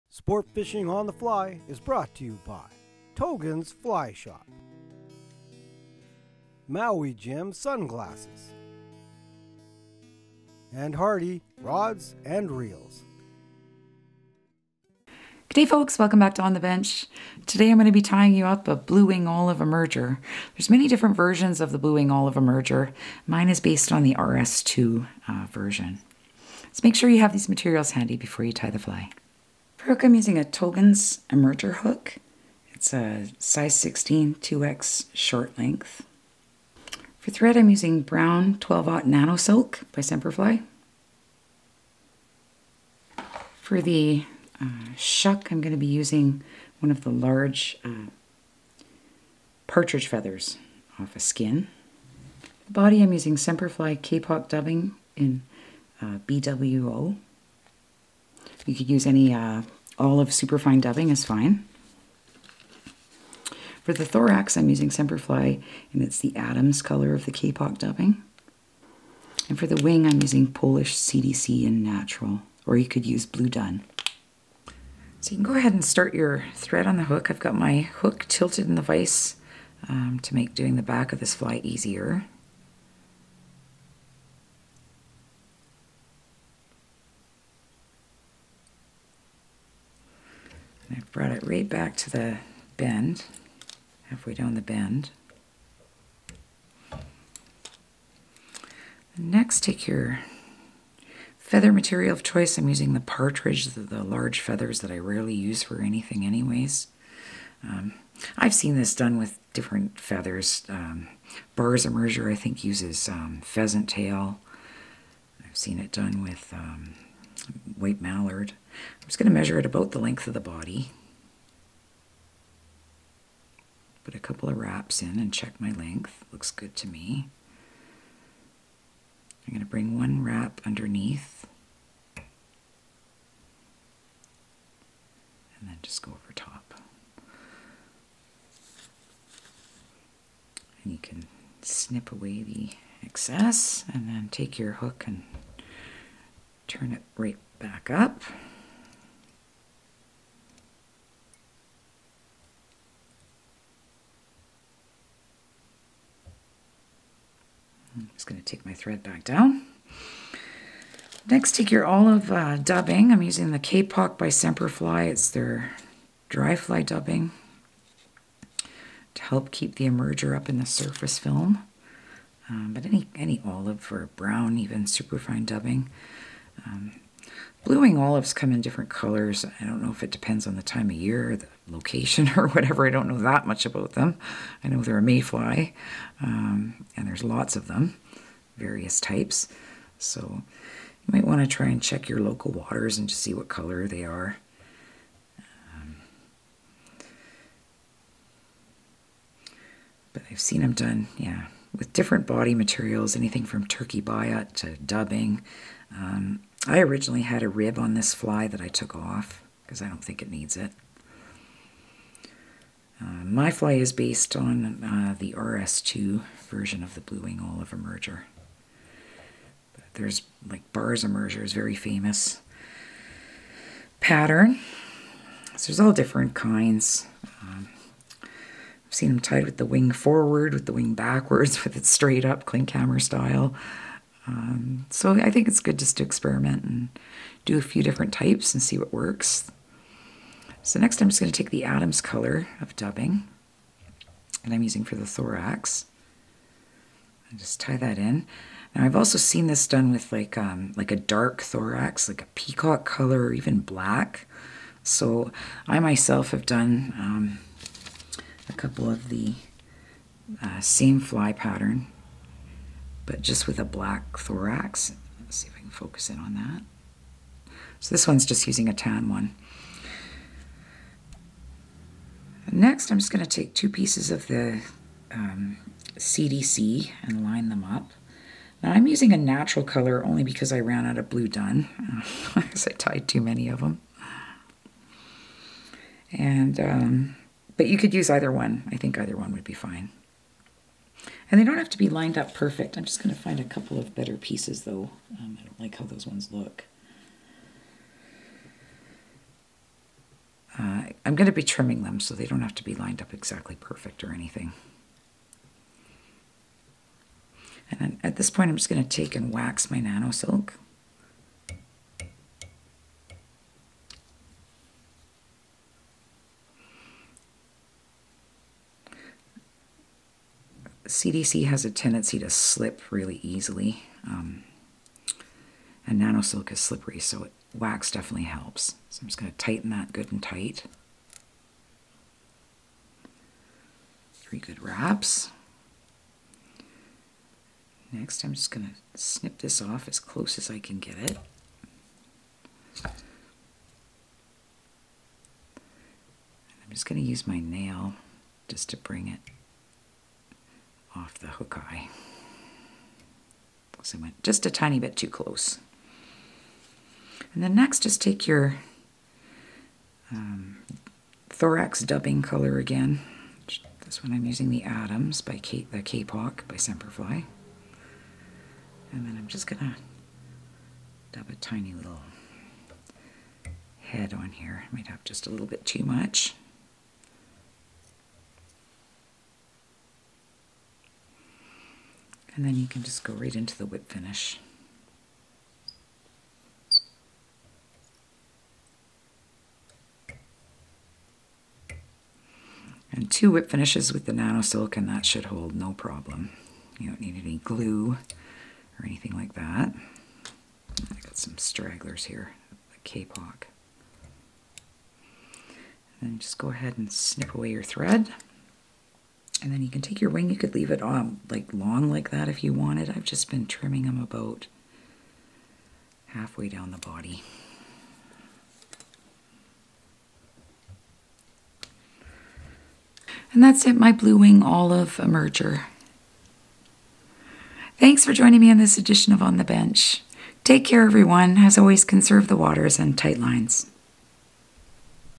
FLY TYING: BWO EMERGER BY DEB PASKALL
Blue Wing Olive Mayflies are a staple food item for trout on moving waters, so Deb Paskall ties up a very effective BWO Emerger Pattern.

List of Ingredients:
Hook Togen's Emerger Size 18
Thread: 18/0 Brown Semperfli Nanosilk
Shuck: Partridge Feathers
Body: BWO Semperfli Kapok
Thorax: Adams Kapok
Wing: CDC Natural